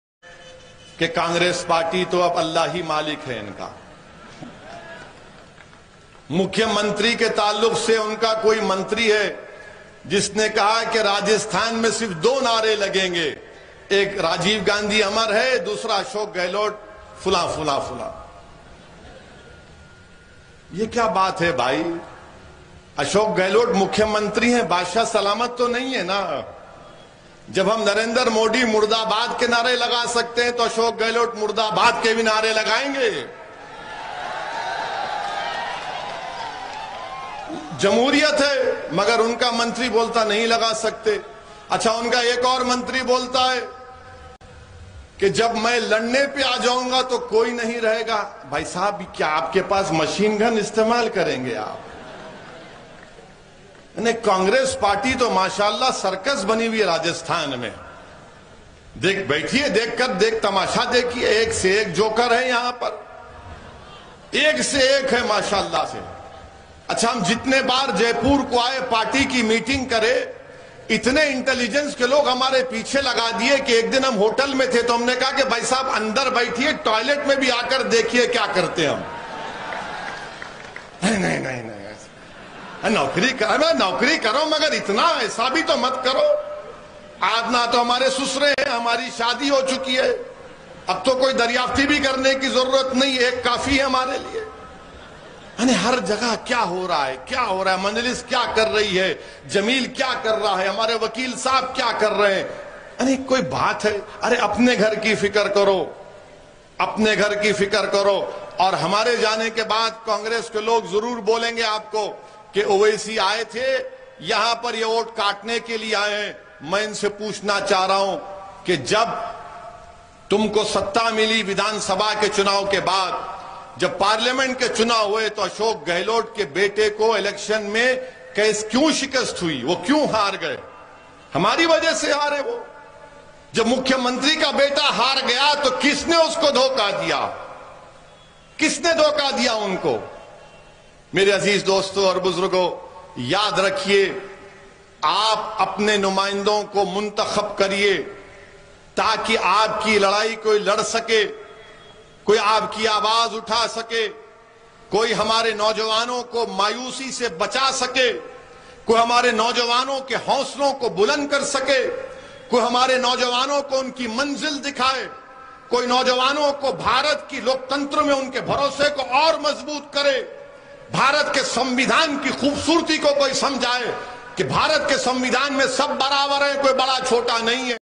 Asadunddin Owesi in Rajisthan। ओवैसी ने BJP से ज्यादा Ashok gehlot or कांग्रेस को लपेटा ।phatehpur।
# Ashok Gehlot # Asadunddinowesi AFSAR FACT

your quaris
asadunddin Owesi in दिनों राजस्थान के दौरे पर है बुध वार को फतेहपुरी एक चुनावी सभा को संबोधित किया। ओवैसी के निशाने पर BJP से ज्यादा cm ashok gehlot ओर उनकी सरकार रही। ओवैसी ने ताना कसते हुए कहा कांग्रेस का तो अल्लाह ही मालिक है। उन्होंने कहा कि कांग्रेस हमे

  वॉट काटने वाले पार्टी बताती है